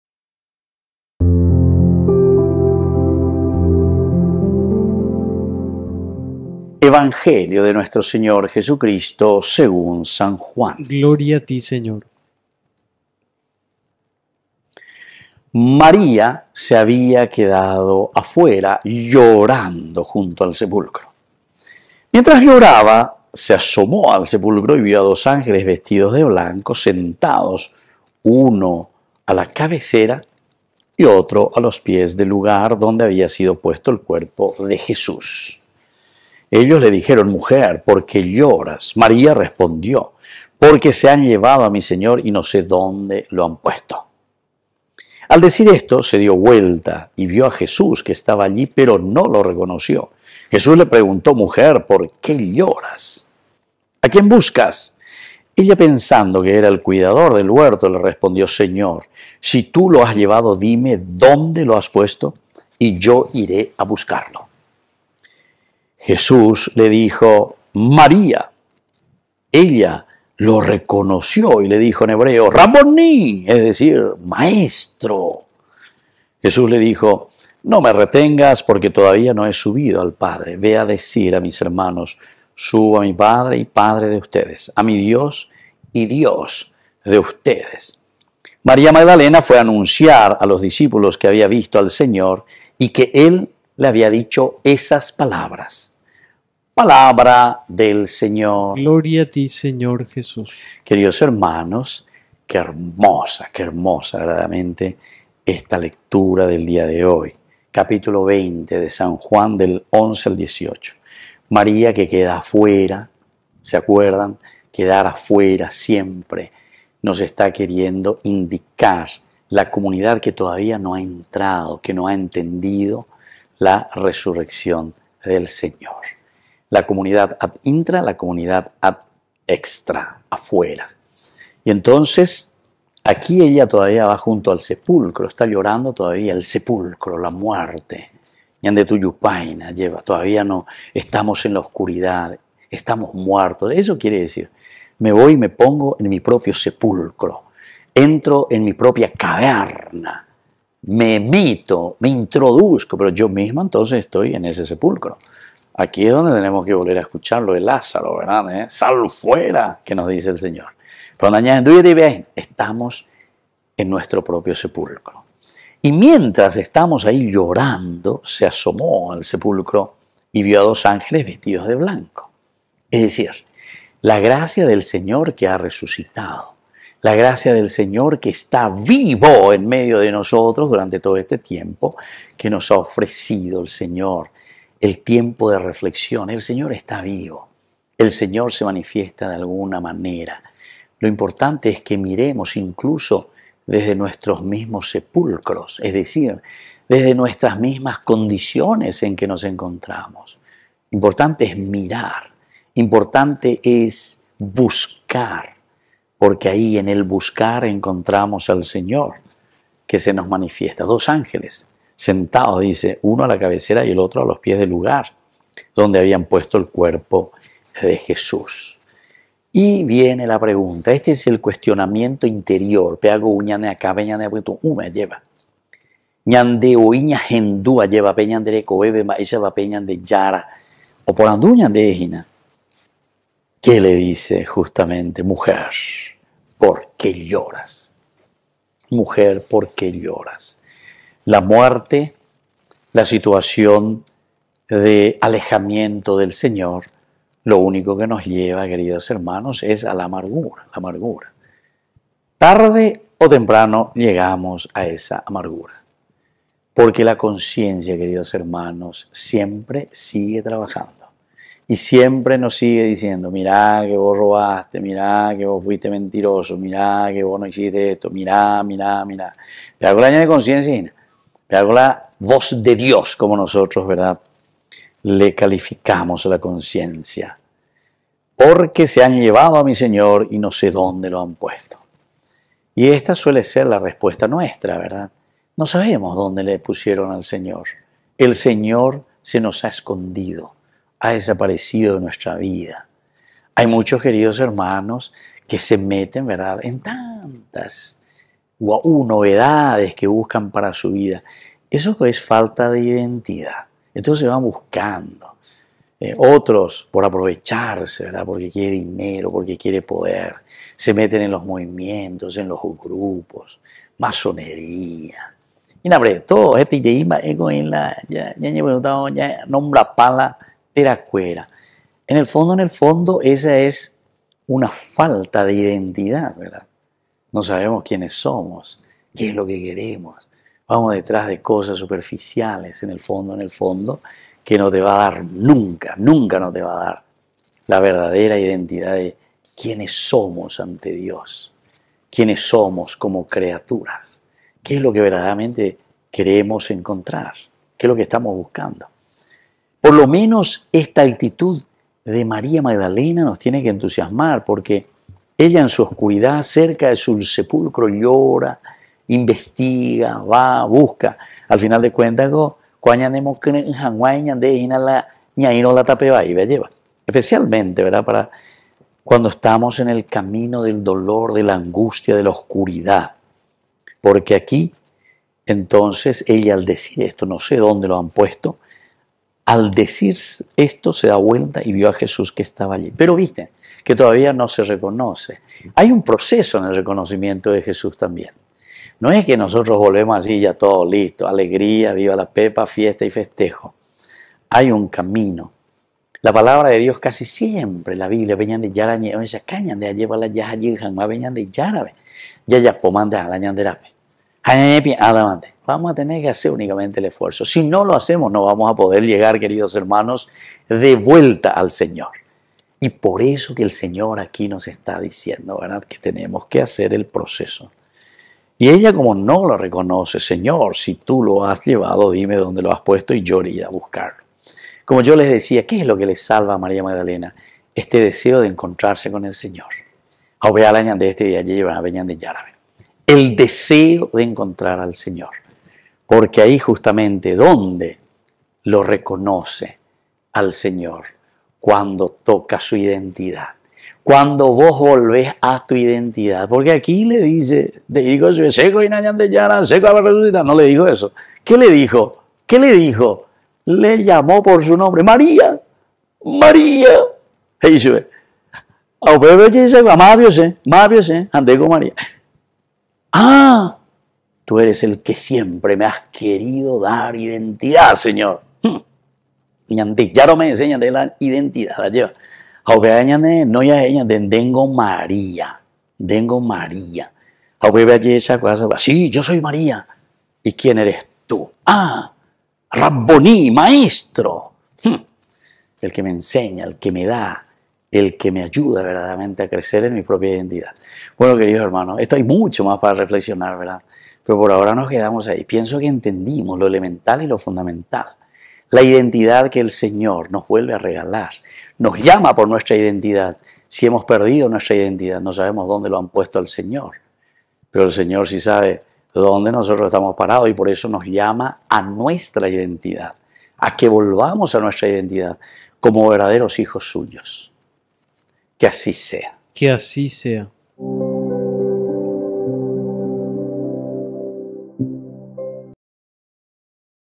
HOMILIA MARTES 11 DE ABRIL DEL 2023
Santo Evangelio según San Juan 20, 11-18

“María se quedó fuera, junto al sepulcro, llorando, volvió la cara y vio allí a Jesús”